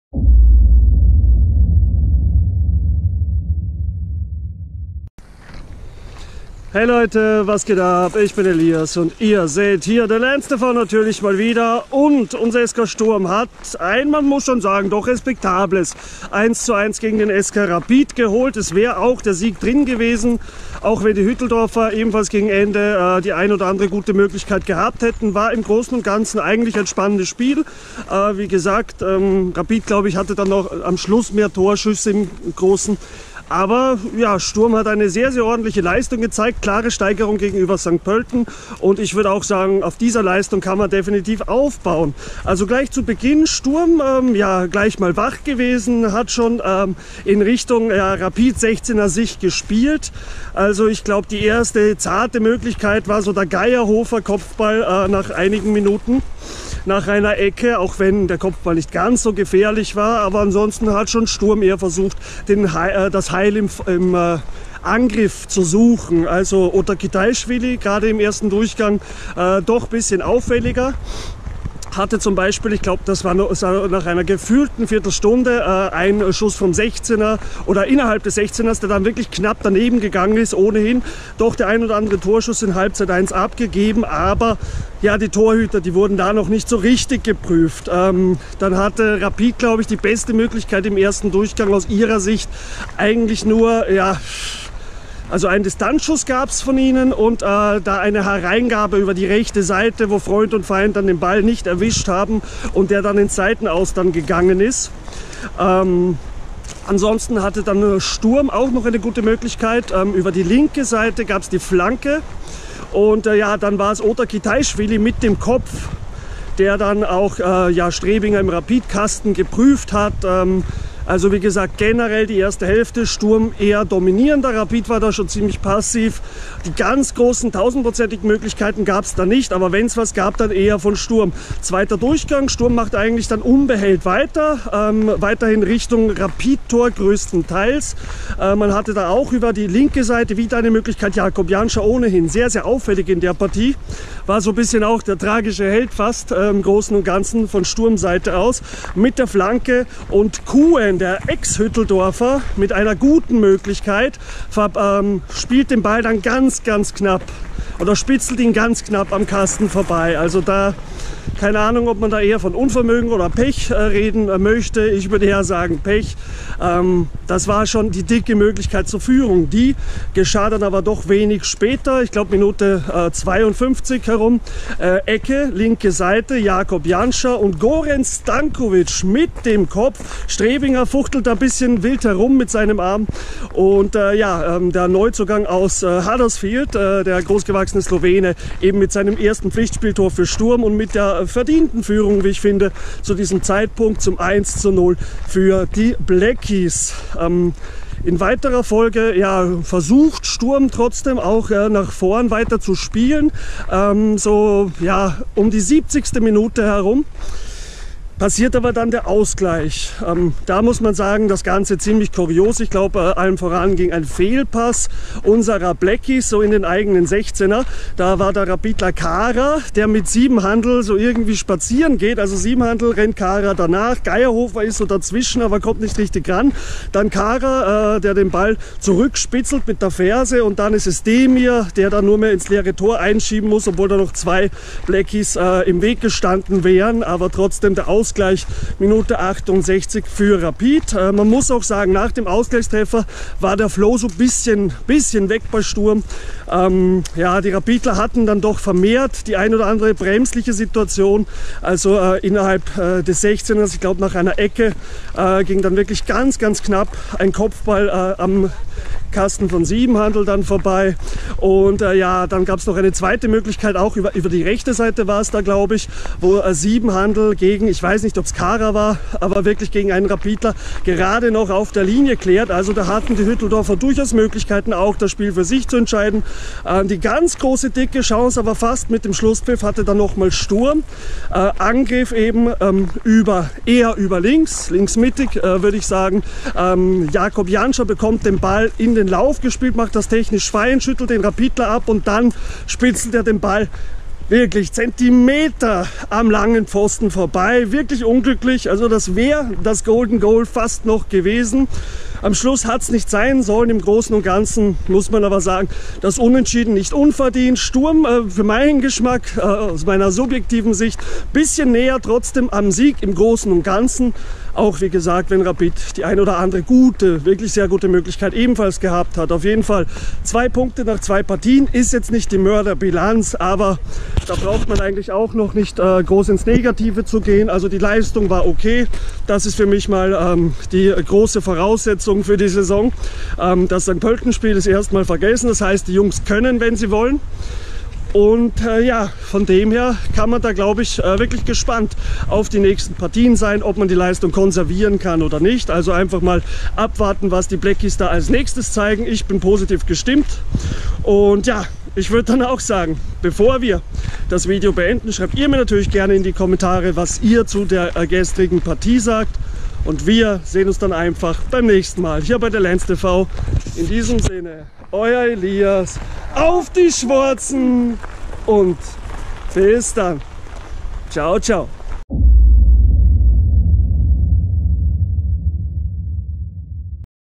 Aufbauend! SK Sturm Graz - SK Rapid Wien 1:1 Nachbericht 2.Spieltag
Das war ordentlich! Die Schwoazen spielen erneut Remis, die Leistungssteigerung war im Vergleich zum Auftakt in St.Pölten deutlich zu merken! Viel Spaß beim zugucken!